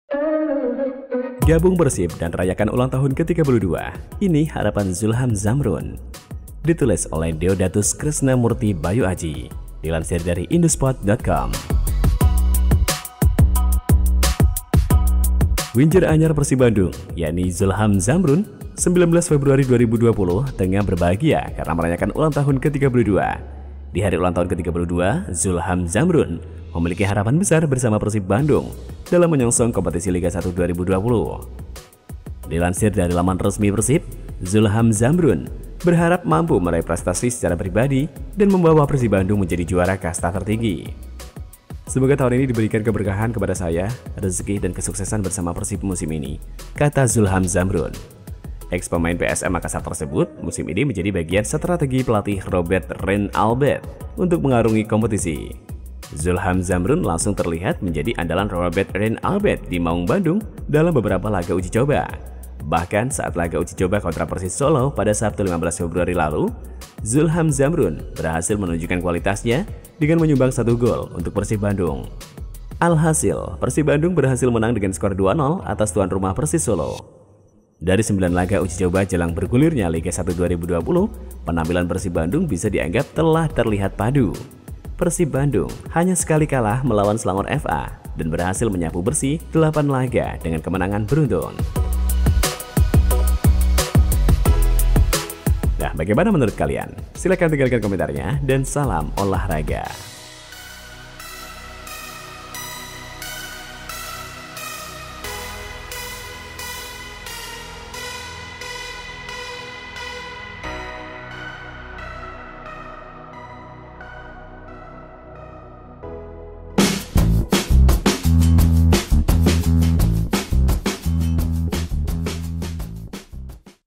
Gabung Persib dan Rayakan Ulang Tahun ke 32, Ini Harapan Zulham Zamrun
Winger anyar Persib Bandung yakni Zulham Zamrun hari ini (19/2/20) tengah berbahagia karena merayakan ulang tahun ke-32.

Di hari ulang tahun ke-32, Zulham Zamrun memiliki harapan besar bersama Persib Bandung dalam menyongsong kompetisi Liga 1 2020.